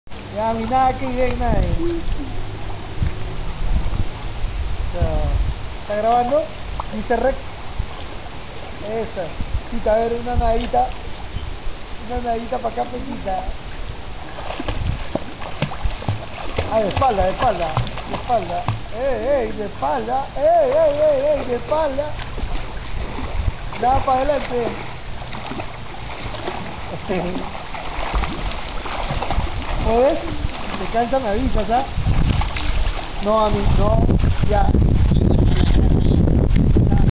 Tita nadando
Mi madre disfrutando de la piscina del Hotel Oasis